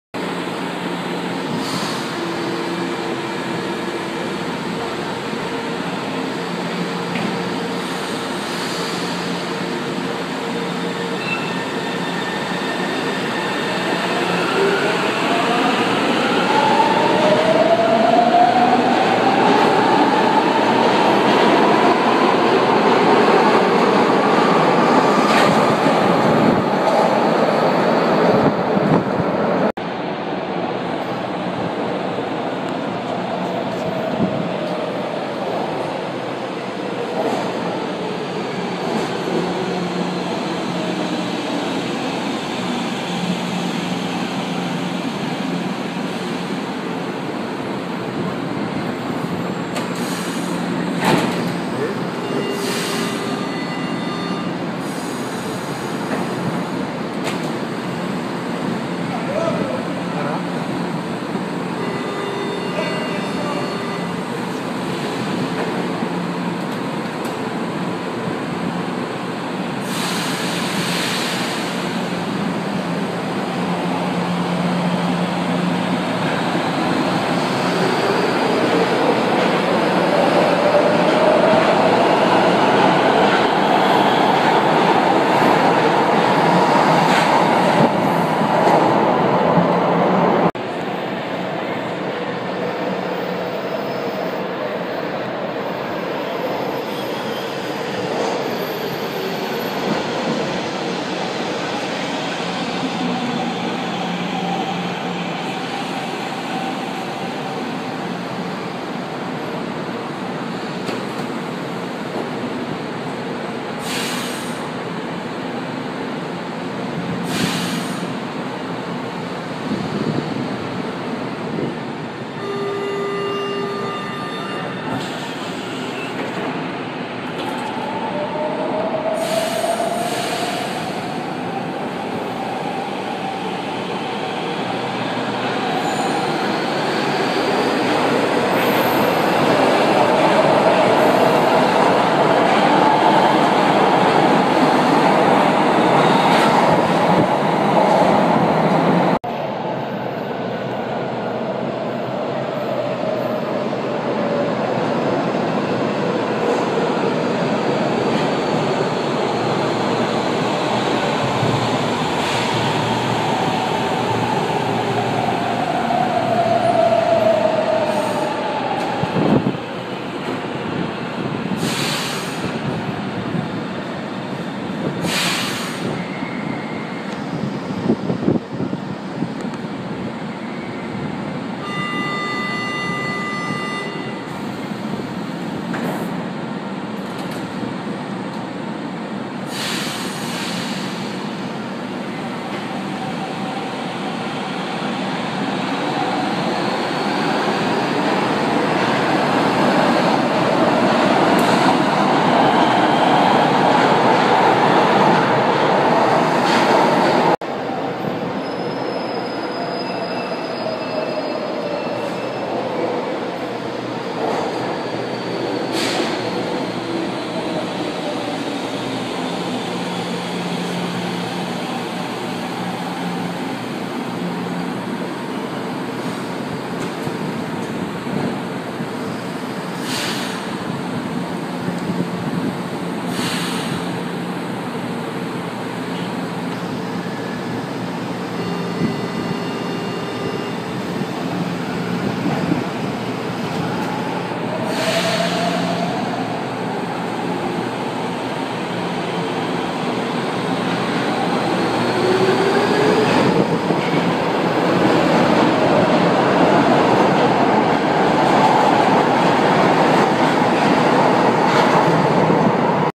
Trens na estação Marechal Deodoro
G24, K12, K03, K20 e K08

Só um trem da Frota G!